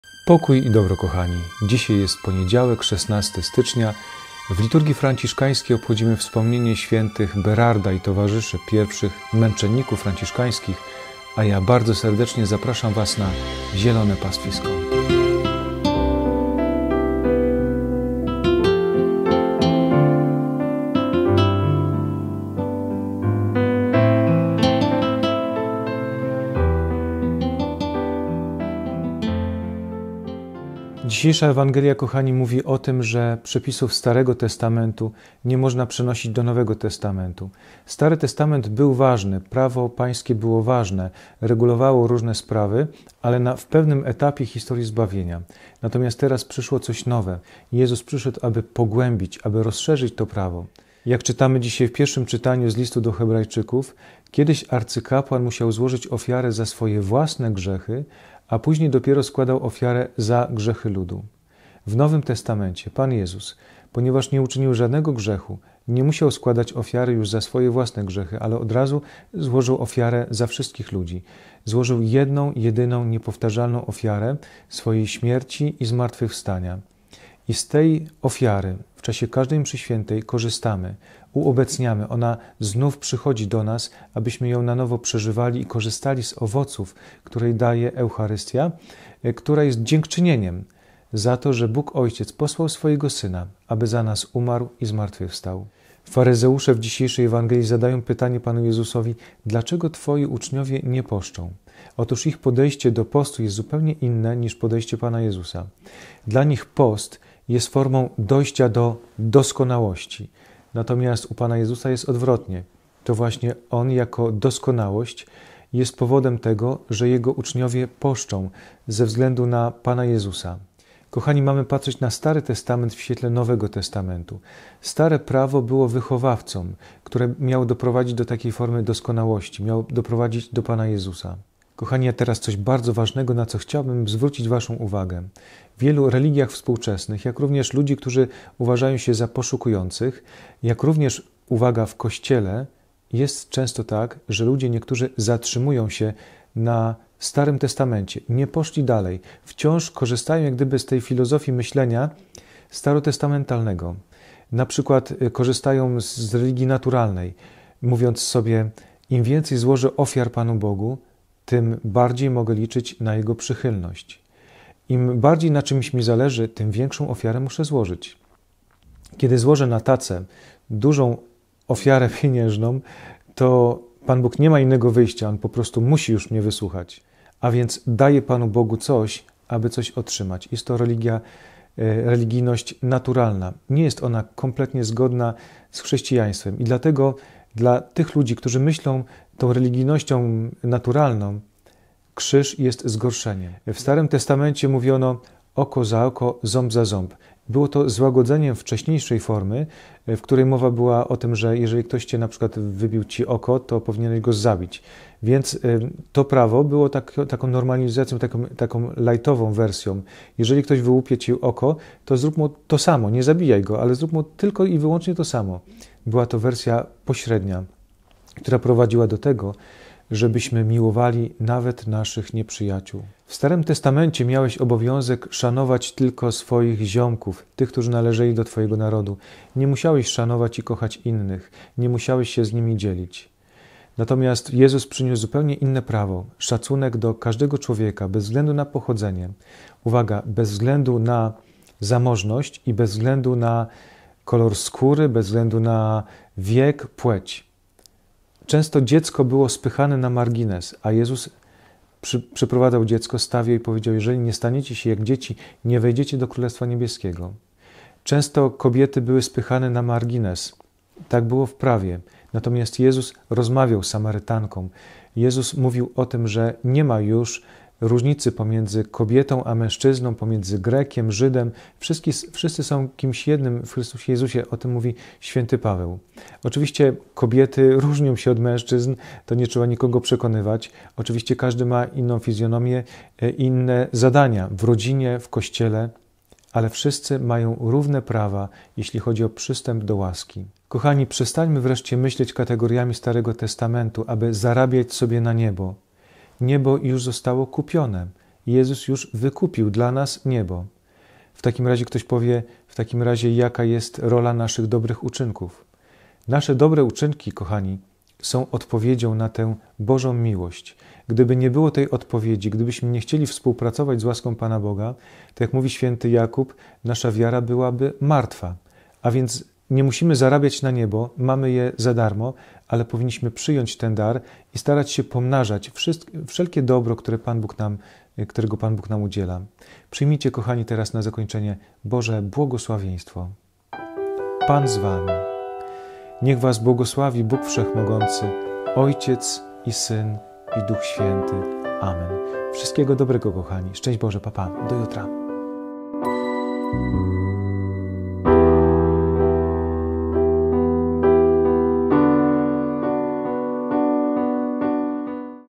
Zielone Pastwisko 16.01.2023 „NOWE PRAWO" [# 1171] - Mk 2, 18-22
Mk 2, 18-22 "Uczniowie Jana i faryzeusze mieli właśnie post. Przyszli więc do Jezusa i pytali: «Dlaczego uczniowie Jana i uczniowie faryzeuszów poszczą, a Twoi uczniowie nie poszczą?» Jezus im odpowiedział: «Czy goście weselni mogą pościć, dopóki pan młody jest z nimi? Nie mogą pościć, jak długo mają pośród siebie pana młodego. Lecz przyjdzie czas, kiedy zabiorą im pana młodego, a wtedy, w ów dzień, będą pościć.
Nikt nie przyszywa łaty z surowego sukna do starego ubrania. W przeciwnym razie nowa łata obrywa jeszcze część ze starego ubrania i gorsze staje się przedarcie. Nikt też młodego wina nie wlewa do starych bukłaków. W przeciwnym razie wino rozerwie bukłaki. Wino się wylewa i bukłaki przepadną. Raczej młode wino należy wlewać do nowych bukłaków»."